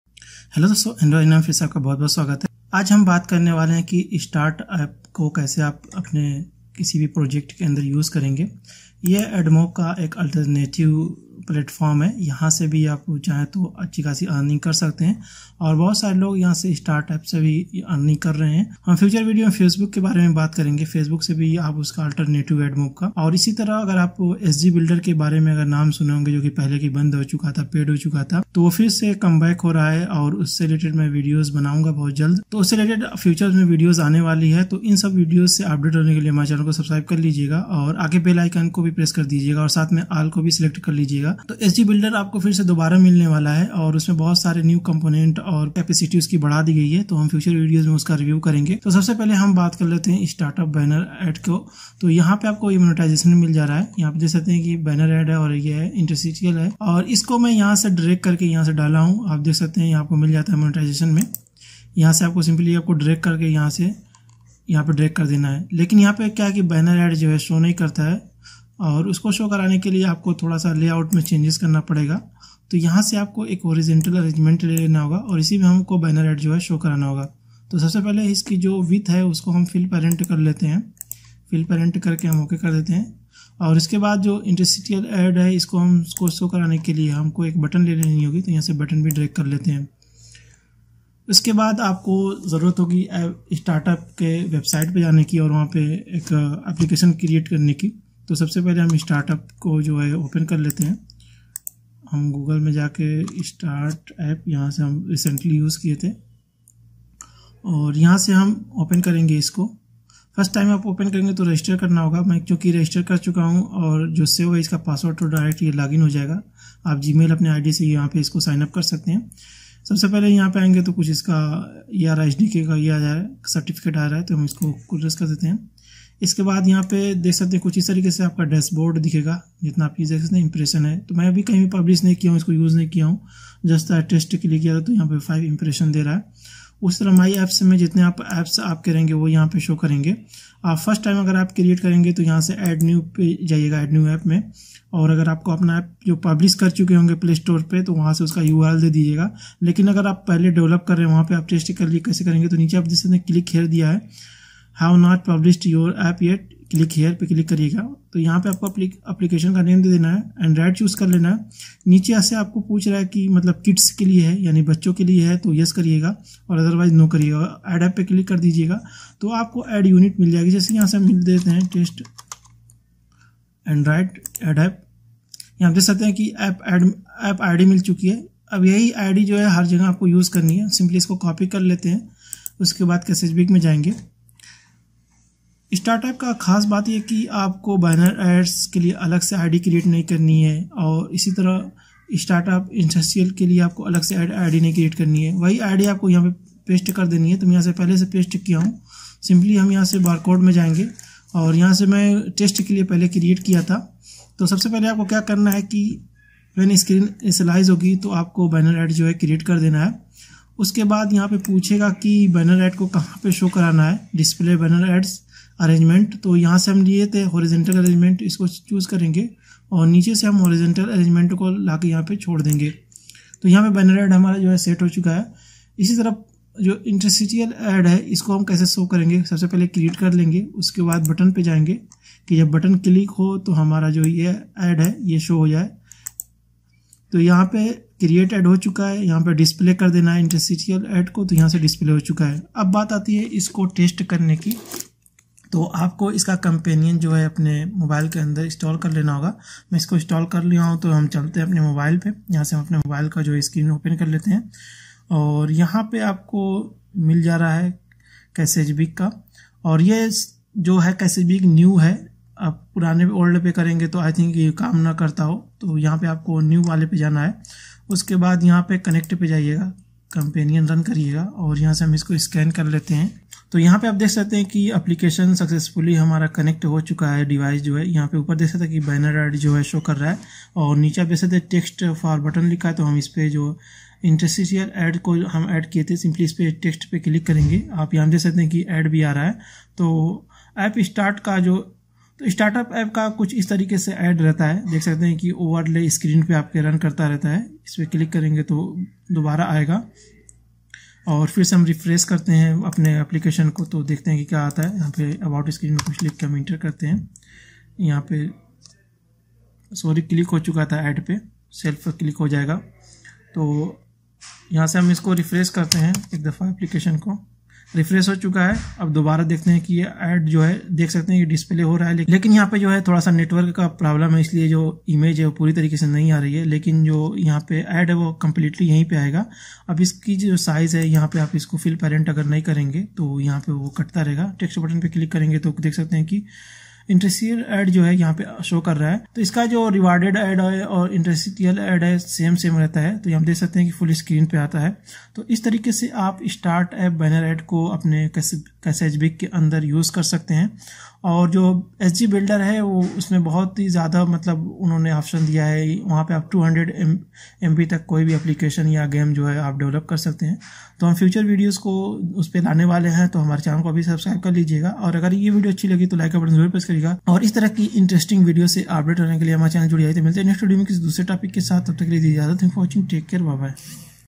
How to use startapp ads in android app development without coding knowledge | Best Admob alternative
How to use startapp ads in android app development without coding knowledge || Admob alternative